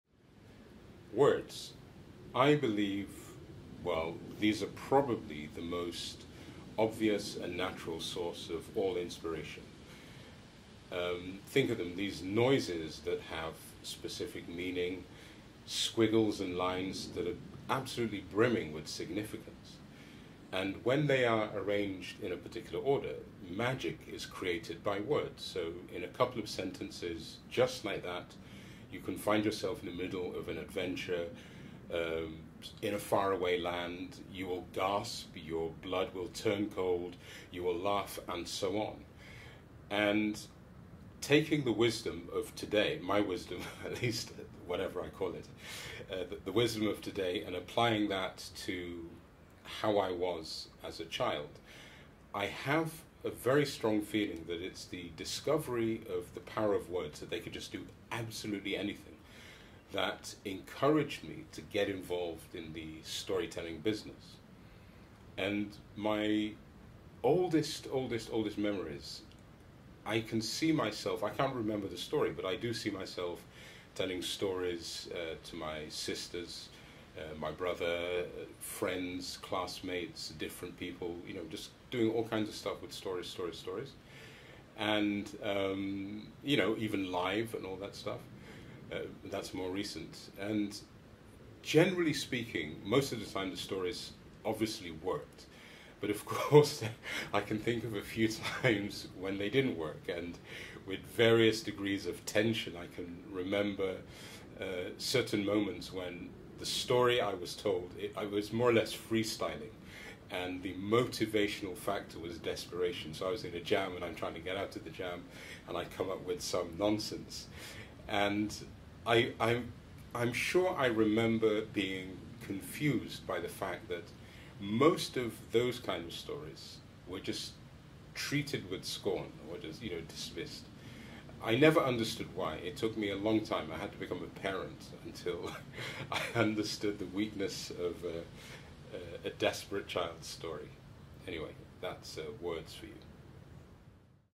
The Author's Note Nº21 - On inspiration (2) - The magic of words
The power of words to create an unlimited number images within the mind